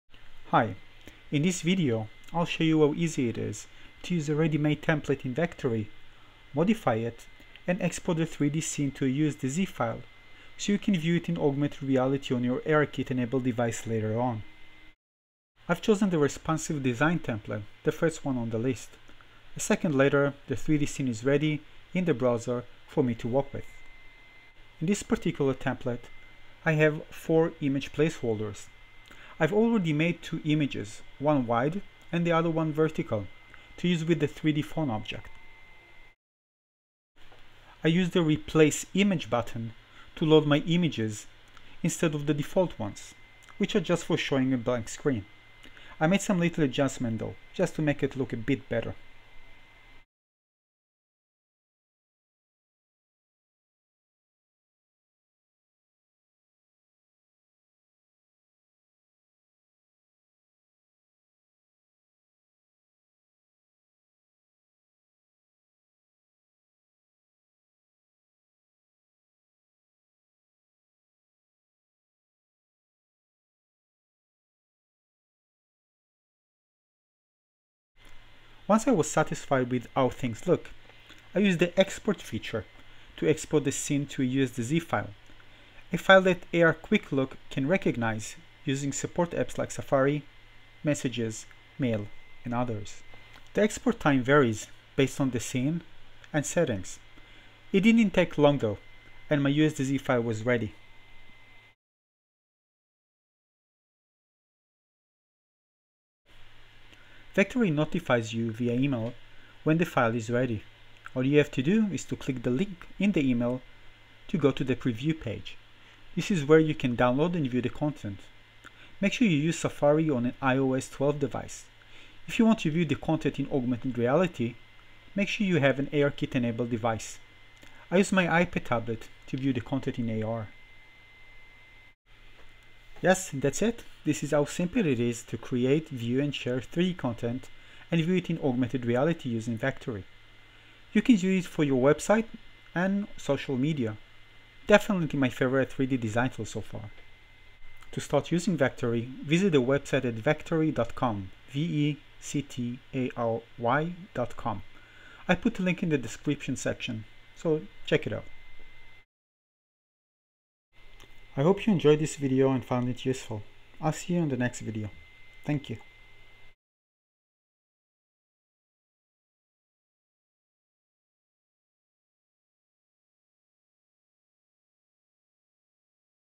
Vectary Guide: Template modification, Export to USDZ, Viewing in AR
In this video, we will take a quick look at how easy it is to create a 3D scene using a default template using Vectary.com online 3D design tool, make some basic modifications, export the 3D scene to USDZ (USD) and then view it on an iPhone or iPad running iOS 12 using Safari and AR Quick Look viewer. If you want to view the 3D scene in augmented reality (AR), make sure you view it on an ARKit-enabled device running iOS 12. I used my iPad 9.7-inch which is compatible with ARKit.

All that process took only a few minutes. If you wanted to know how can you easily share 3D content for other (or yourself) to view in augmented reality on the iPhone or iPad, now you know how to do it.

Good luck. If you have questions, please don't hesitate to ask.